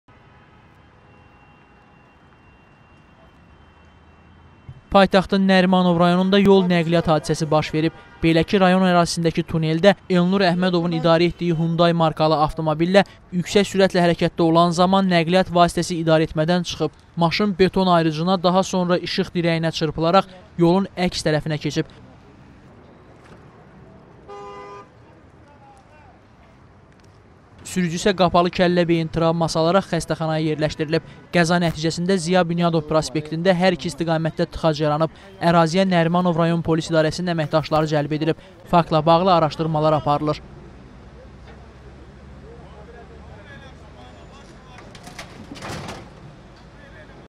Avtomobilini tuneldə aşıran sürücü
Yüksək sürətlə hərəkət edən maşın tuneldə qəza törətdi